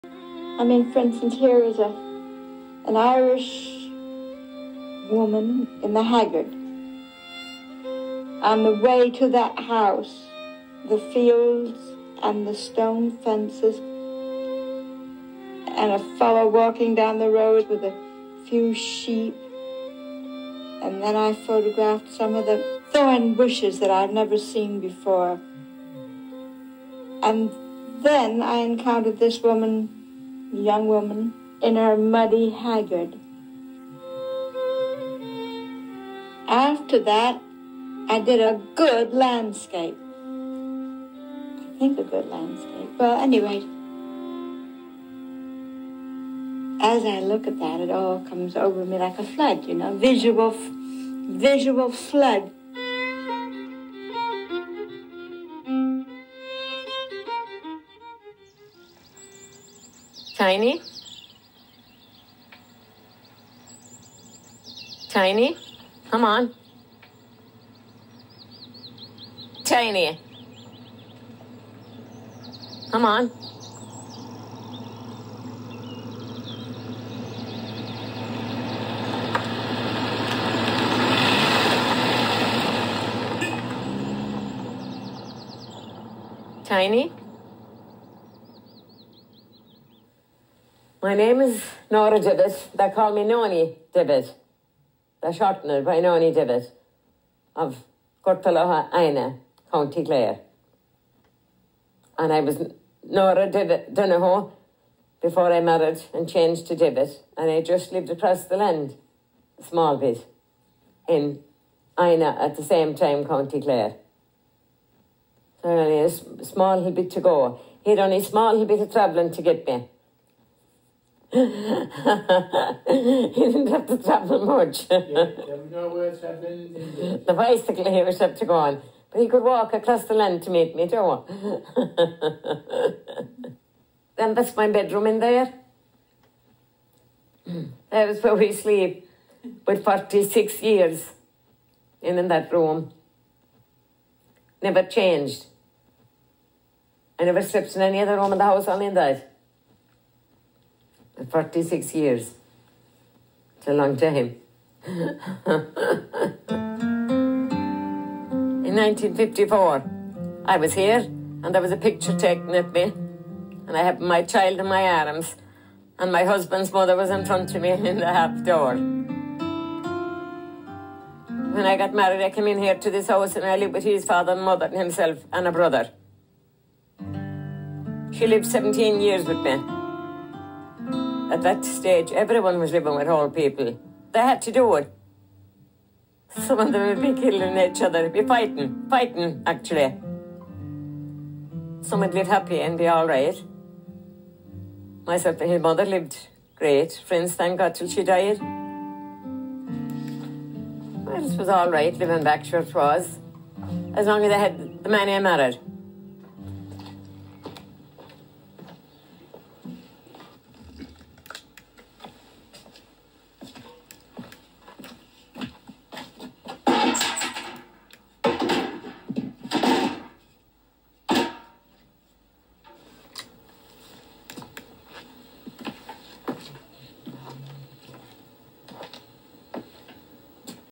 Dorothea Lange remembered Nora Devitt
Nora Devitt recalls the visit to Co Clare in Ireland by photographer Dorothea Lange in 1954, except from a 2002 documentary by Anne Heraty, Dierdre Lynch and Gerry Mullins. The documentary is based on Gerry Mullins' 1996 book "Dorothea Lange's Ireland." (Elliott & Clark, 1996) (ISBN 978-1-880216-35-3 ISBN: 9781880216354 ISBN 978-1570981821) from the Dorothea Lange archive